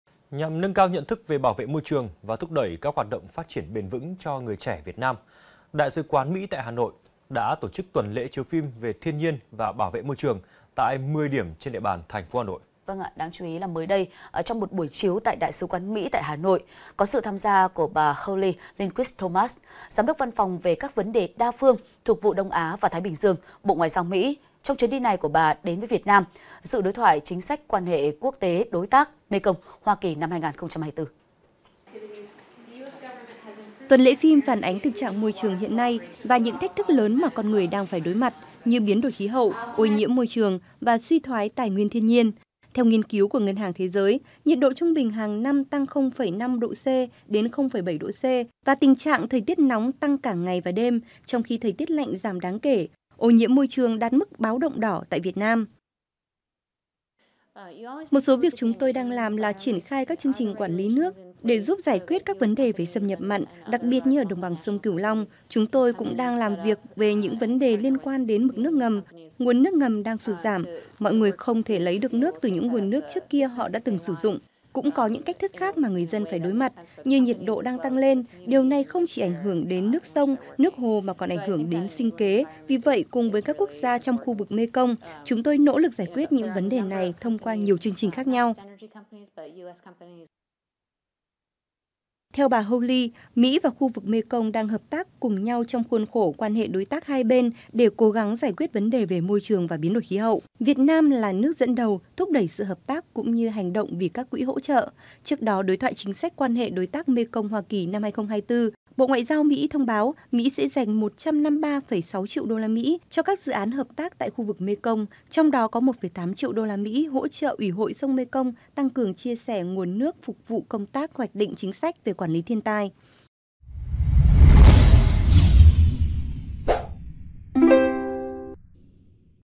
Đại sứ quán Mỹ chiều phim về bảo vệ môi trường cho thanh niên Việt Nam | VTC14
VTC14 | ĐẠI SỨ QUÁN MỸ CHIỀU PHIM VỀ BẢO VỆ MÔI TRƯỜNG CHO THANH NIÊN VIỆT NAM
Nhằm nâng cao nhận thức về bảo vệ môi trường và thúc đẩy các hoạt động phát triển bền vững cho người trẻ Việt Nam, Đại sứ quán Mỹ tại Hà Nội đã tổ chức tuần lễ chiếu phim về thiên nhiên và bảo vệ môi trường tại 10 điểm trên địa bàn TP Hà Nội.